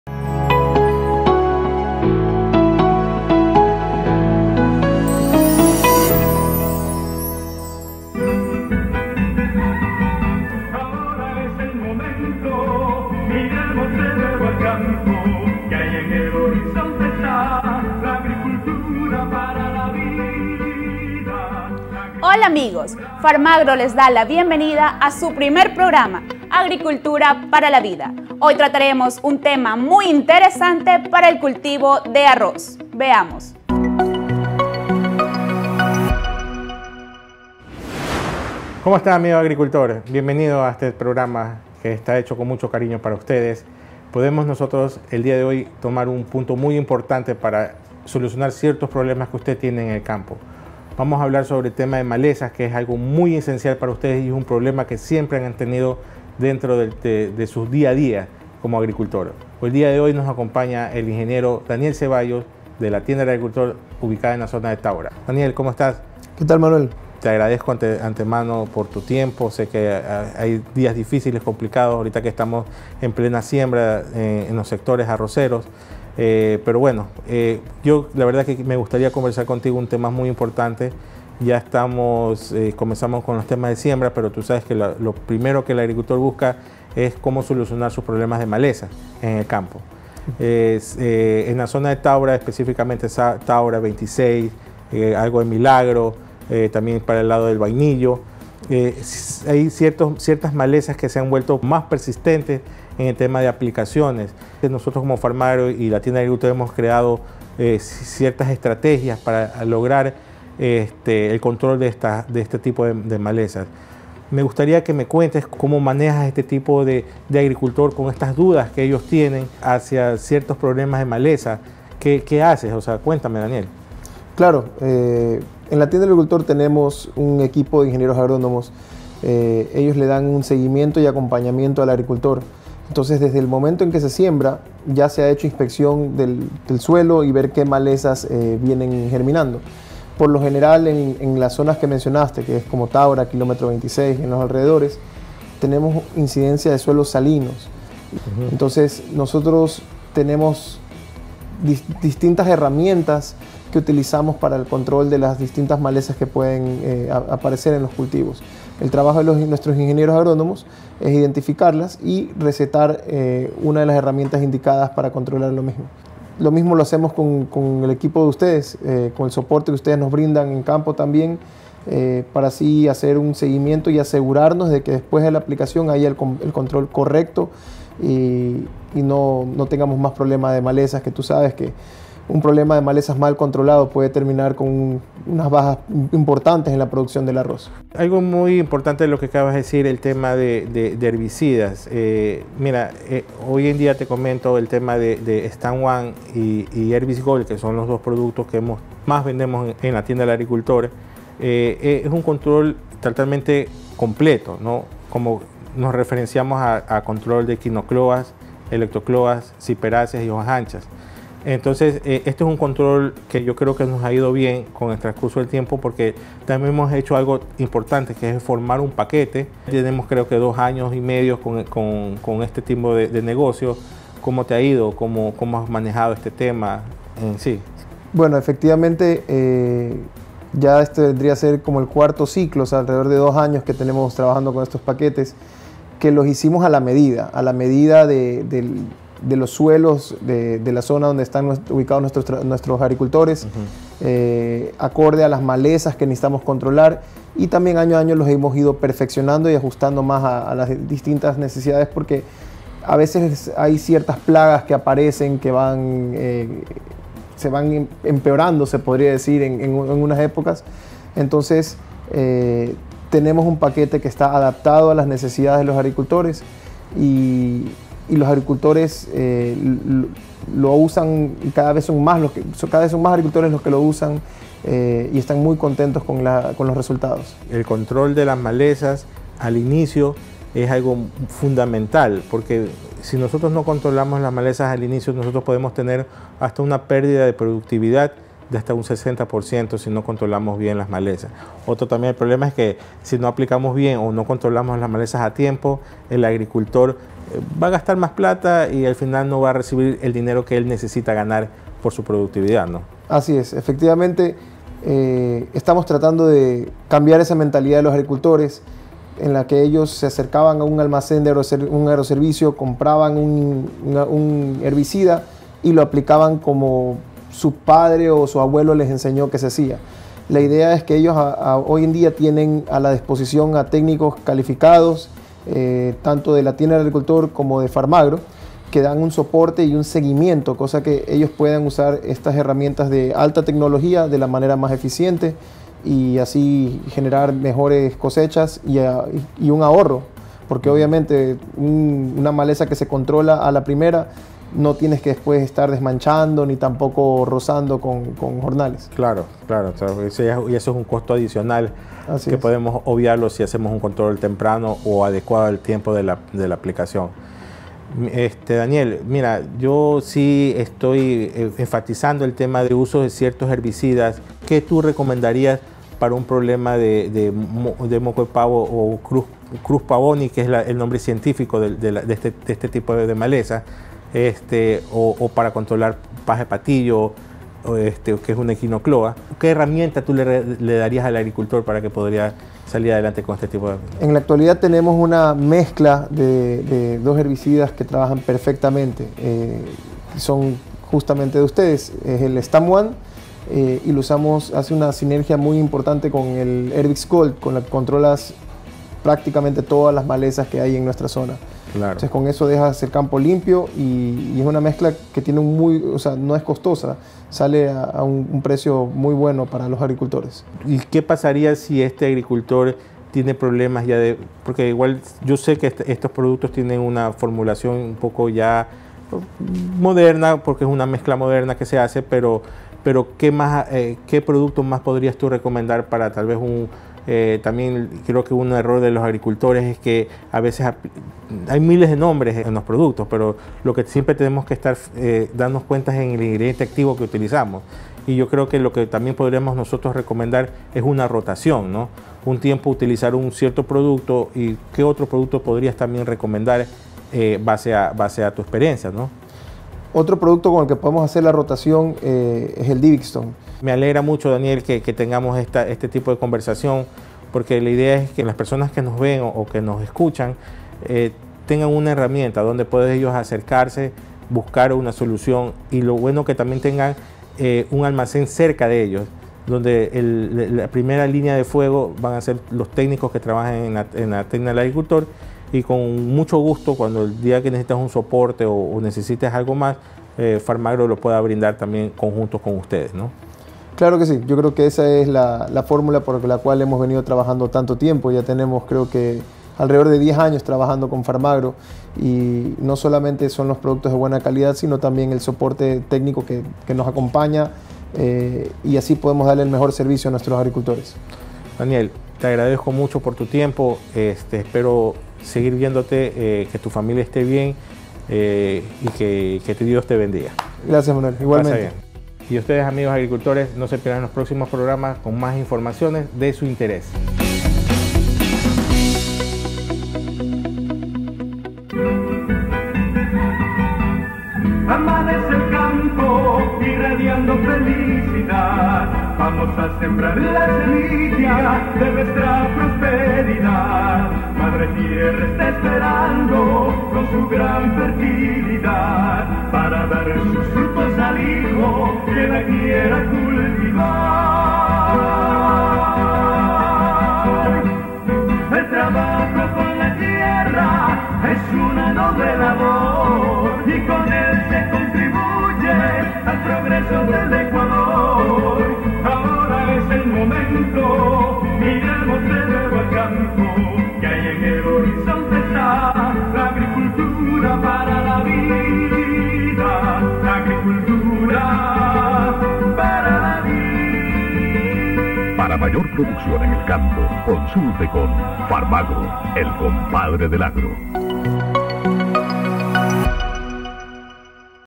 Agricultura para la vida - Cultivo de Arroz
Bienvenidos a nuestro primer programa "Agricultura para la vida",
en este primer capítulo hablaremos sobre el cultivo de arroz, las principales  malezas y como combatirlas al inicio y a lo largo de la siembra.
Nos acompaña nuestro cliente el Ing. Daniel Cevallos desde Taura junto a nuestro técnico Manuel Yúnez.